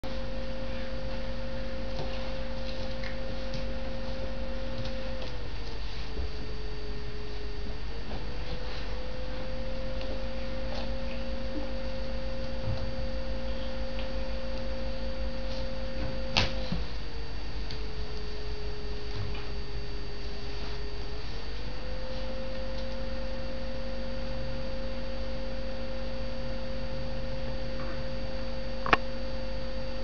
里面是什么？ 1
这是7月5日的影片。
他很喜欢打开抽屉然后将里面的东西全都丢出来。 所以低层的抽屉有得用婴孩锁锁住了， 没有婴孩锁的用透明胶粘着，上面的他打得开但是够不着就算了。